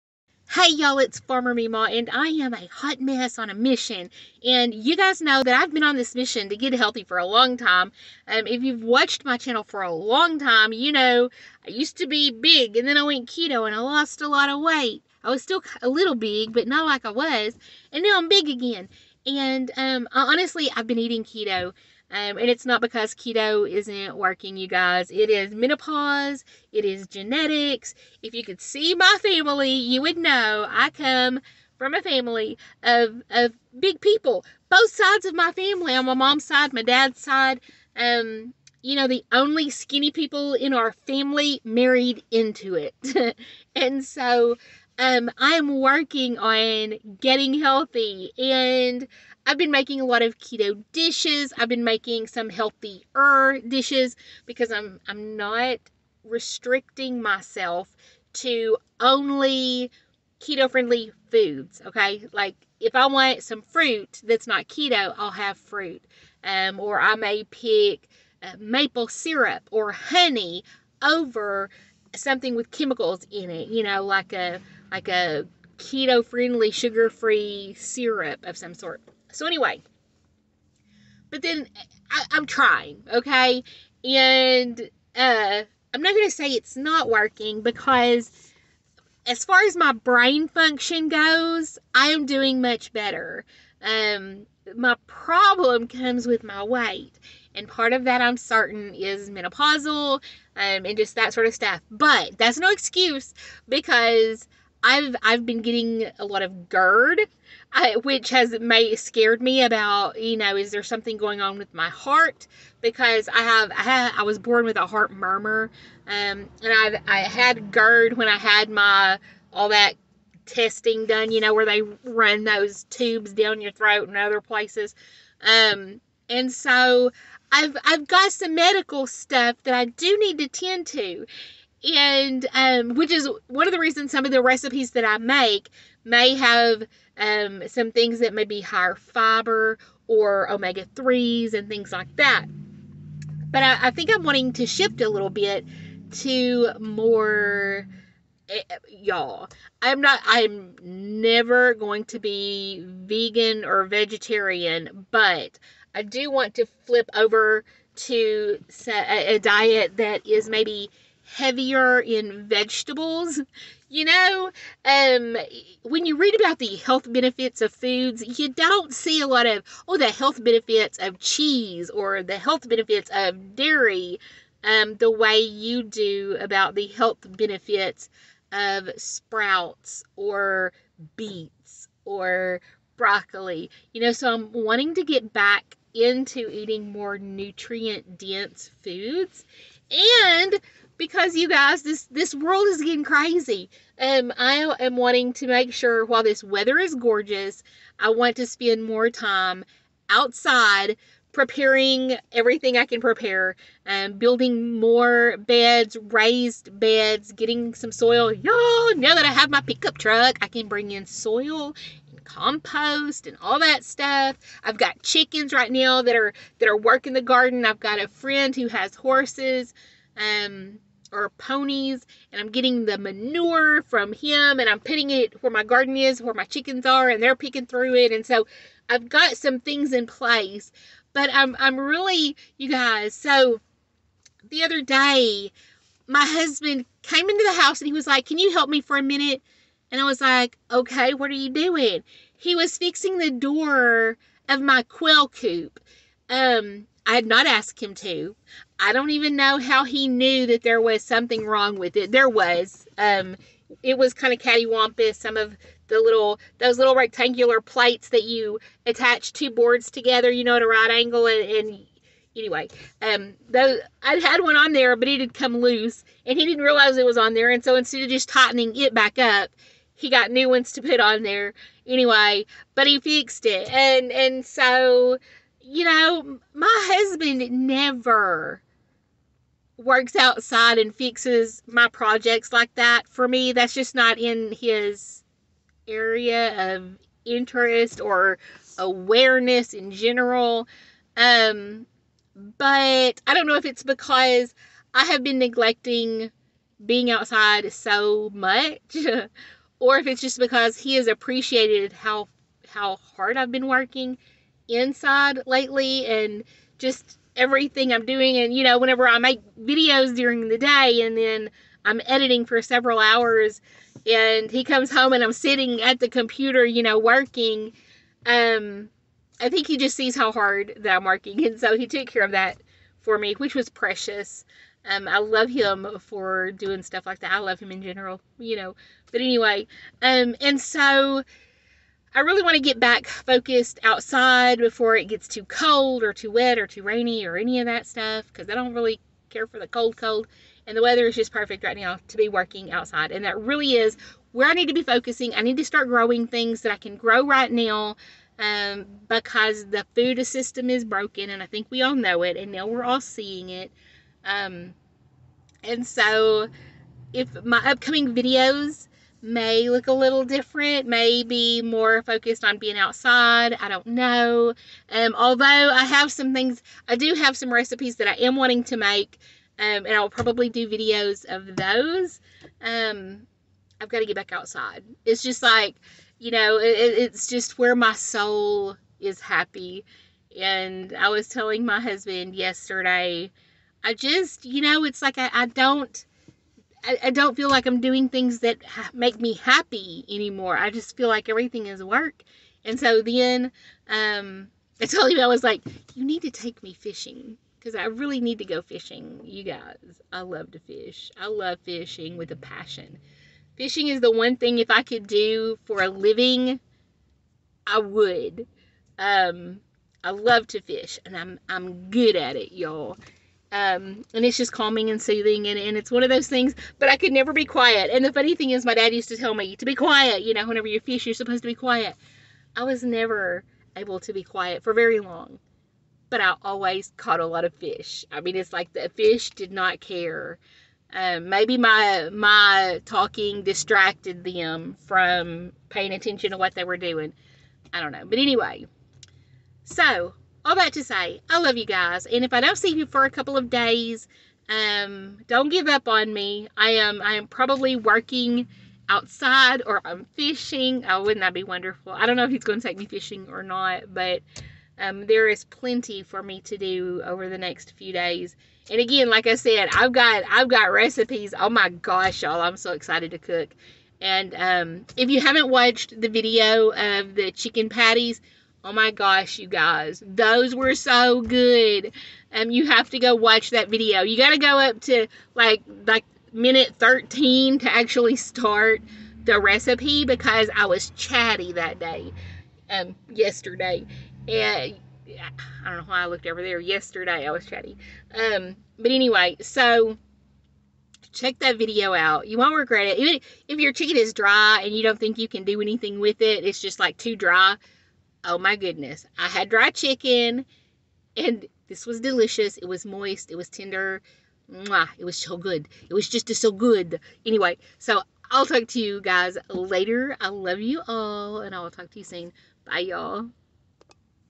Fitness Friday - menopause, genetics, weight loss and garden chat
Hello friends!!  Today I just wanted to talk about focusing on what is important, and doing what needs to be done.  Whether we are dealing with genetics, menopause or unhealthy choices, being in control of your health and food supply has never been more important.